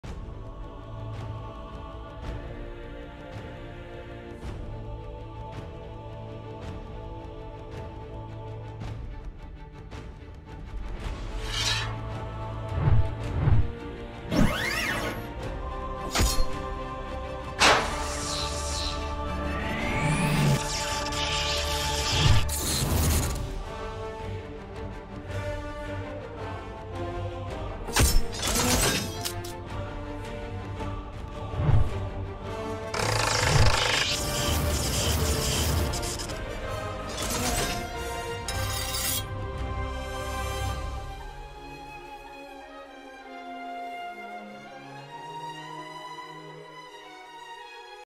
Rathian Charge Blade, Dear Lutemia
I've been playing a lot of Monster Hunter Rise: Sunbreak and wanted to do one of the many weapons that they had. I have a bit of a history hunting pink Rathian in the older games, but since we don't have those in this version, I went with a normal Rathian weapon. I chose the charge blade mostly because I really like the transformation and its axe attack, and I also wanted to practice texturing something more organic, but still wanted it to be a weapon of some sort. Regarding those points, what's better than a MH weapon? I am really happy with the end product.

Programs: Autodesk Maya, Adobe Substance 3d Painter, Arnold and After Effect